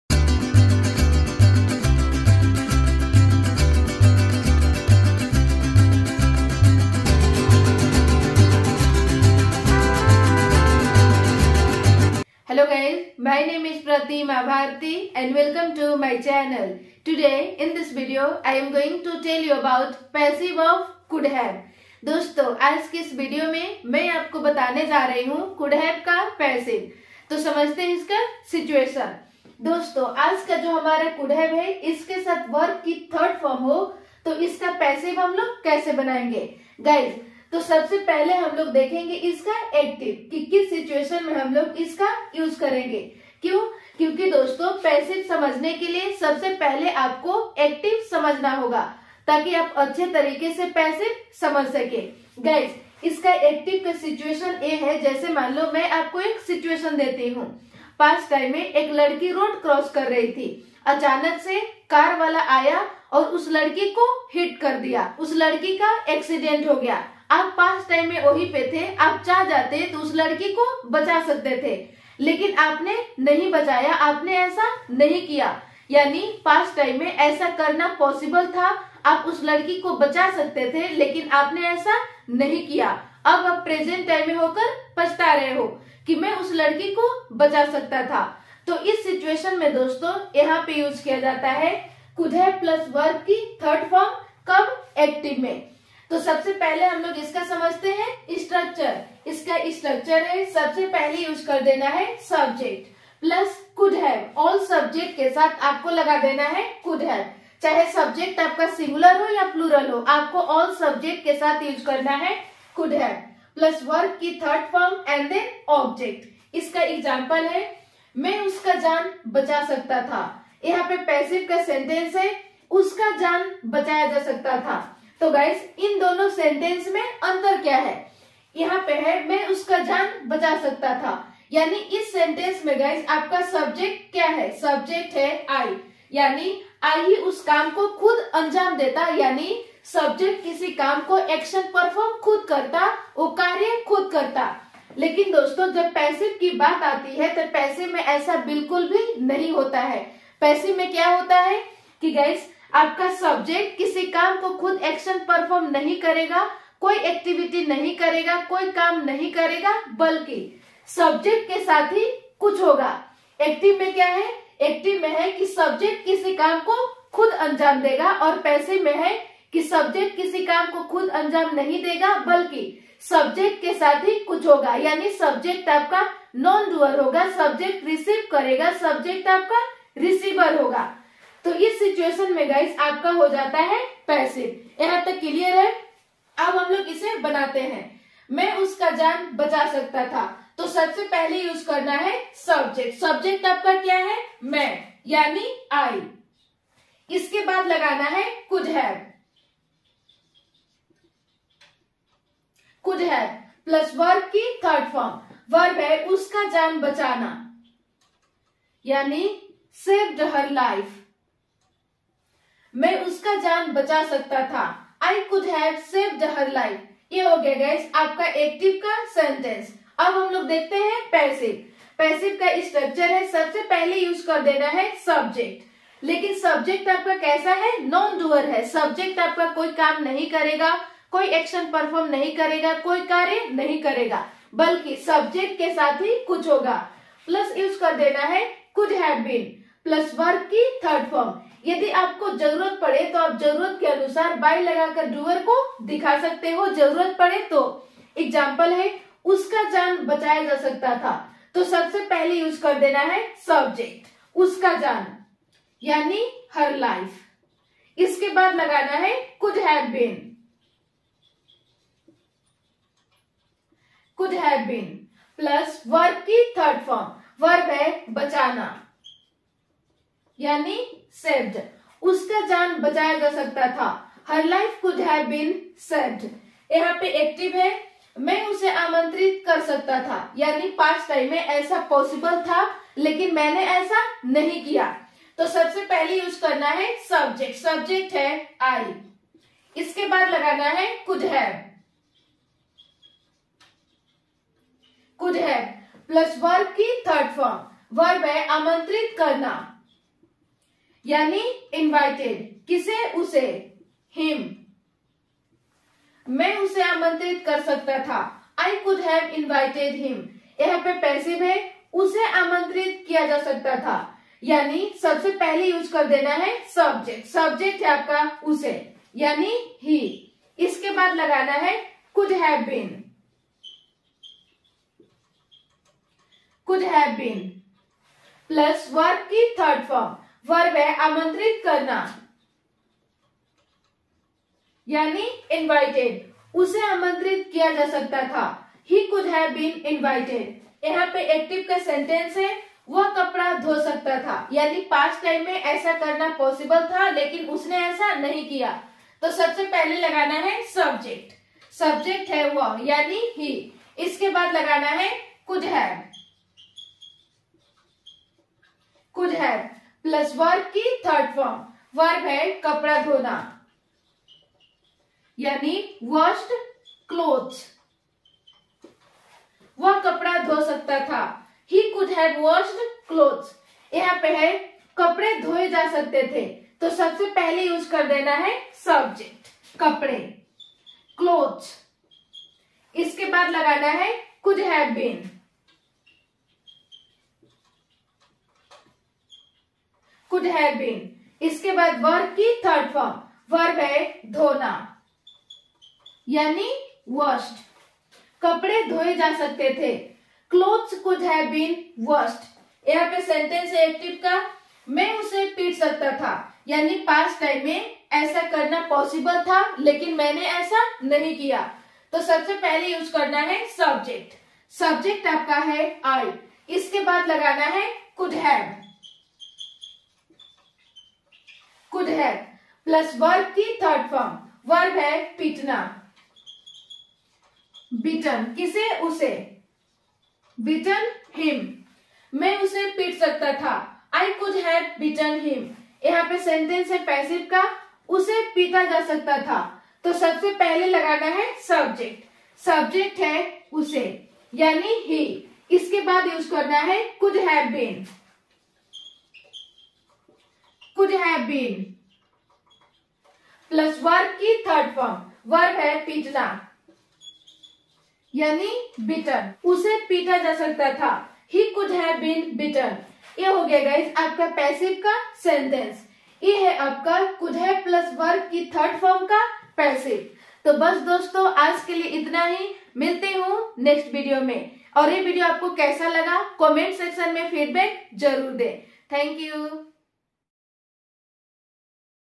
Modal Passive || Could Have +V3 (Past Possibility) || Spoken English
Hello guys,

In this video I am going to tell you about could have+V3.

Pratima Bharti vlogs channel link!